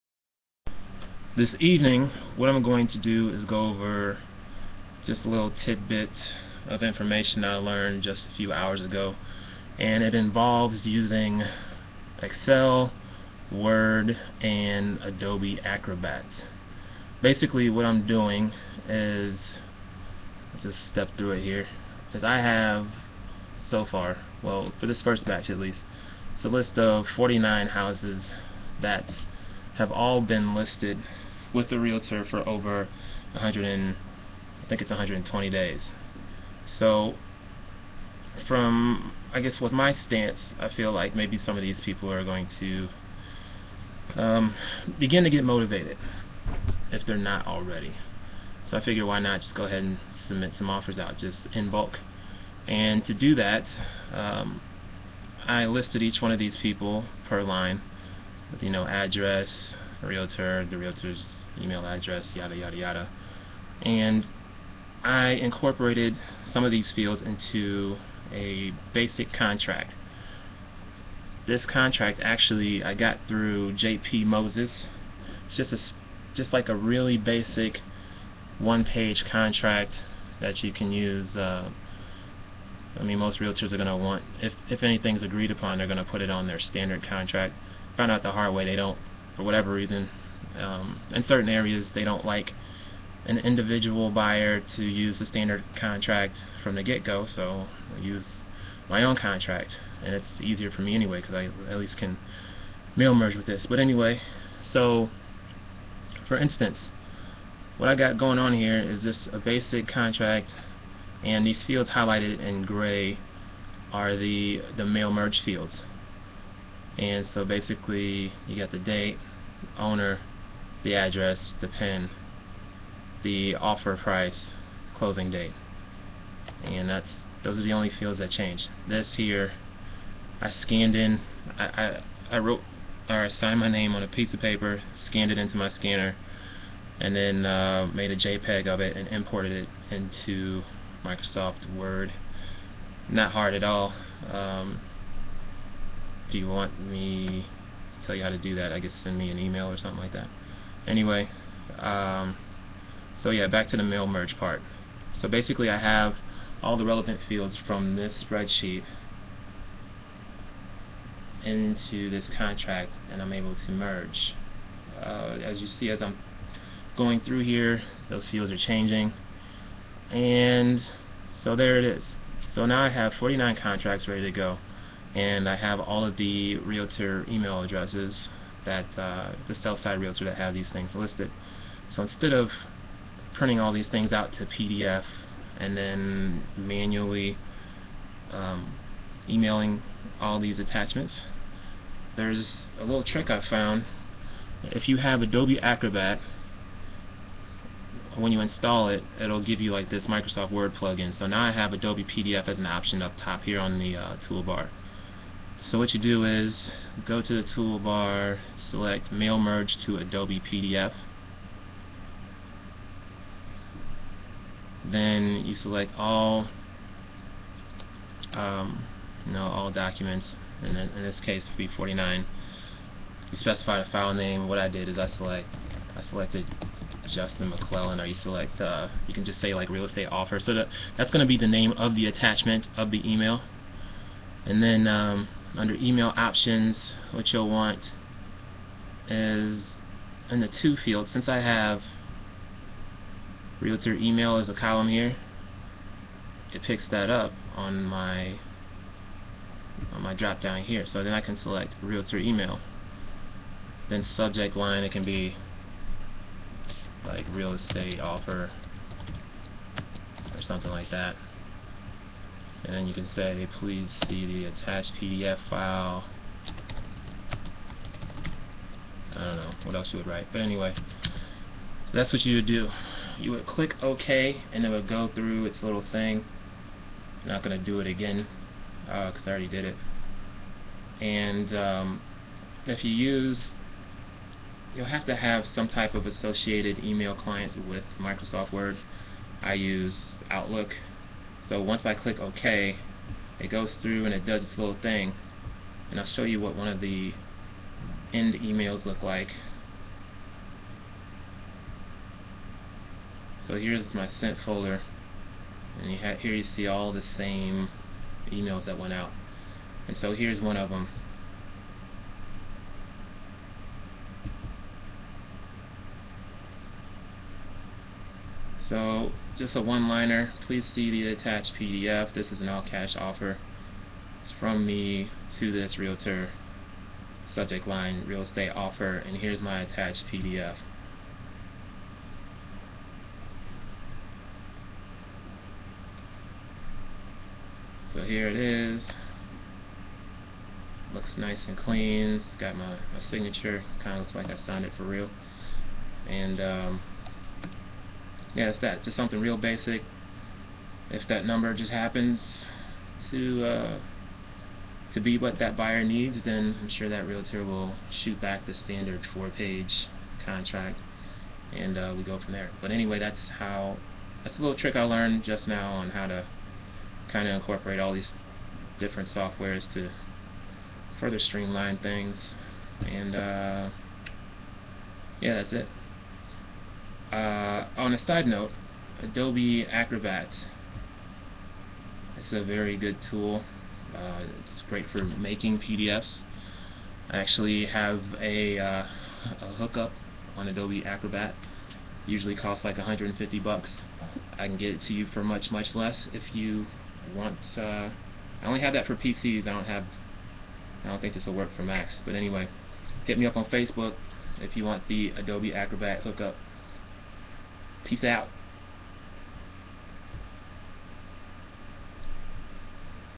Mail Merge to Adobe Acrobat to Email - How To - Real Estate Wholesale
I provide a tutorial on how I used Mail Merge to Adobe PDF to email.